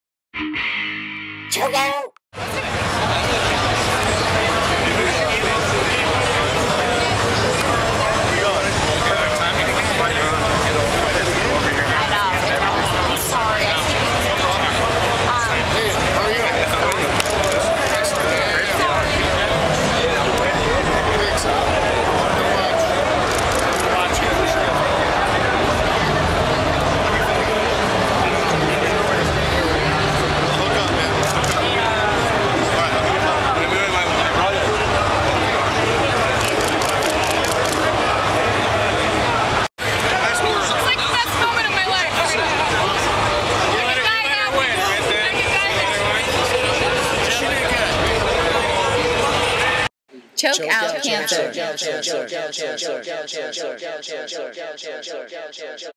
E3 Expo with UFC's Champ Fabricio Werdum and Dan Herderson
ChokeOuT Cancer was at the E3 Expo at the Staple Center in LA and got this super cool video of Dan Henderson.  Dan Henderson is a super wrestler who is one of the best MMA fighters ever.  He's fought and been champion for Pride FC, Strikeforce and the UFC.  Dan Henderson was at the E3 Expo helping to promote the EA Sports MMA video game.  Dan has a big UFC fight vs. Suga Rashad Evans coming up soon.  Go Hendo